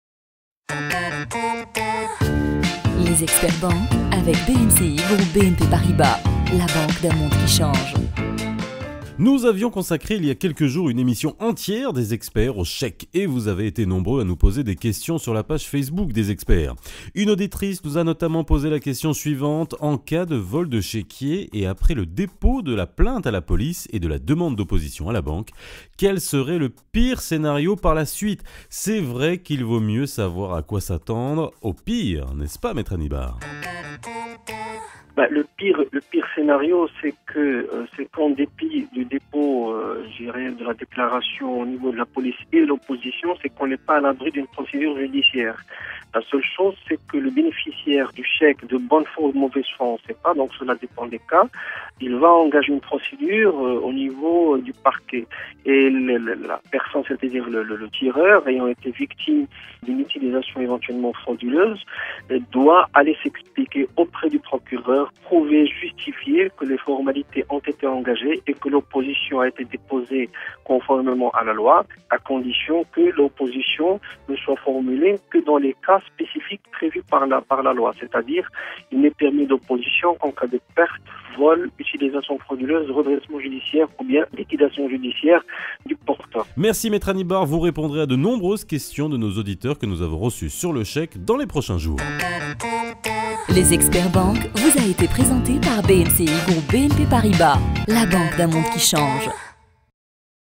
LES EXPERTS BANQUE - Perte ou vol de chéquier
À quoi s’attendre lors de la perte ou du vol de votre chéquier ? La réponse par notre expert banque, dans cette capsule Experts Banque.